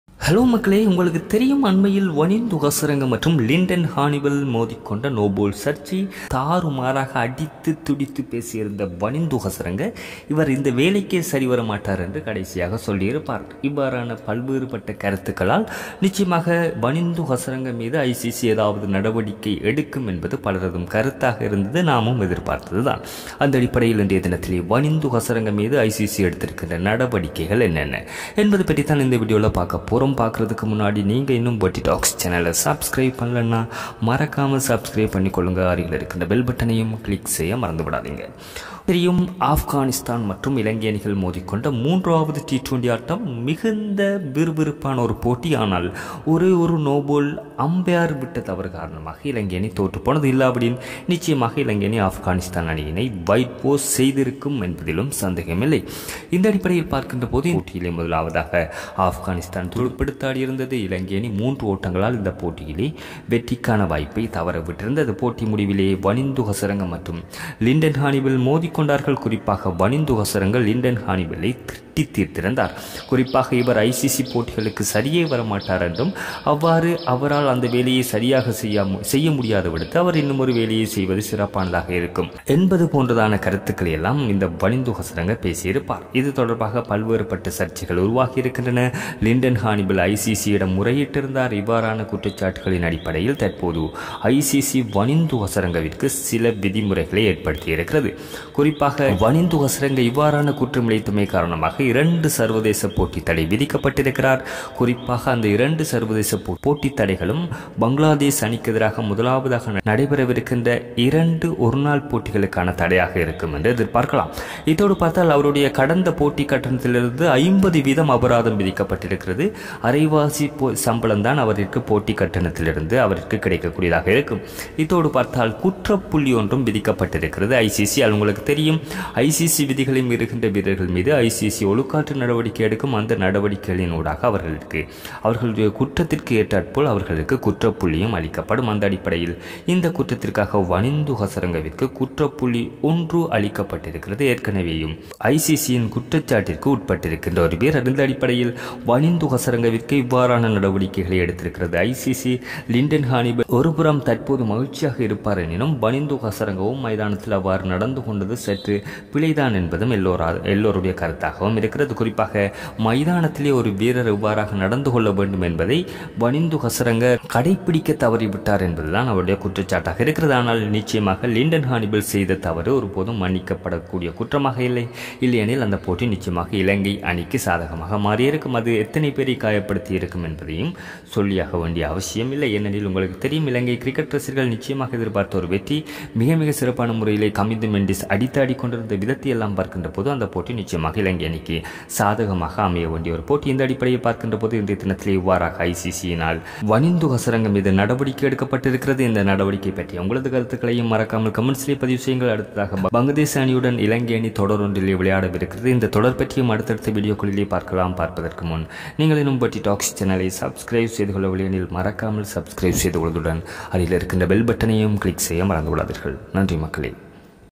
ஹசரங்கவிற்கு போட்டித்தடை ICC எச்சரிக்கை | Hasaranga Hannibal Noball Issue | SL vs AFG
This Channel is talk about the other side and Unknown Facts.Enjoy our Videos and do Share Your Comments.